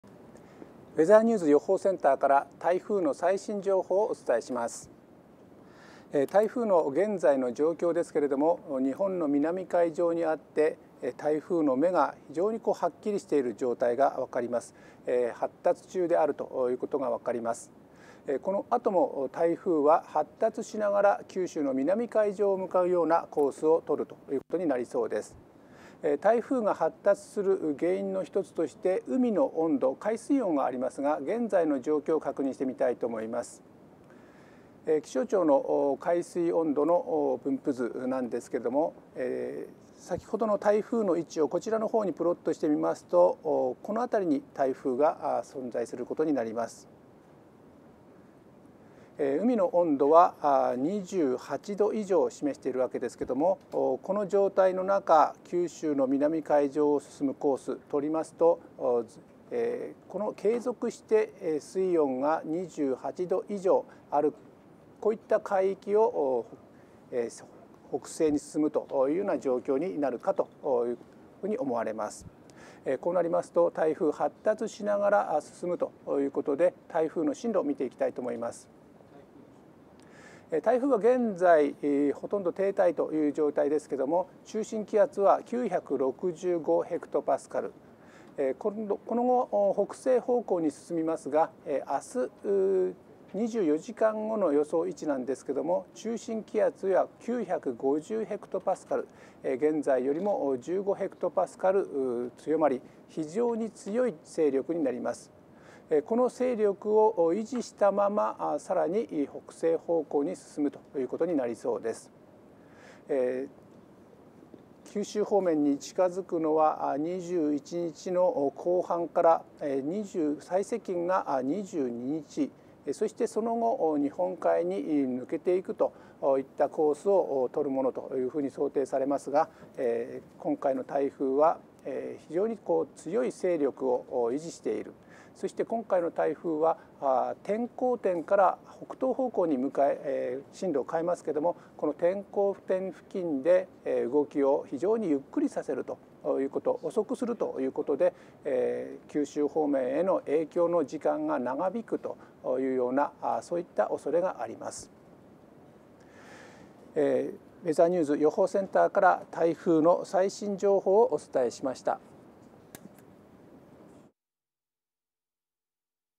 【強い台風19号】明日にも非常に強い勢力に 来週九州へ接近
強い勢力の台風19号（ソーリック）は、父島の南西の海上でほとんど停滞しています。今後は西北西に進みながら、明日19日(日)には非常に強い勢力にまで発達する見込みです。
その後は北寄りに向きを変え、21日(火)から22日(水)にかけて西日本に接近または上陸する恐れがあります。西日本の太平洋側は、台風が接近する前から大雨となる可能性があります。また、台風は暴風域を伴って強い勢力のまま近づくため、暴風雨となる恐れも。
とりうる進路にはまだ東西の幅があるため、できるだけ最新の進路予報を確認の上、備えを進めてください。

※ソーリック：ミクロネシアの言葉で伝統的な部族長の称号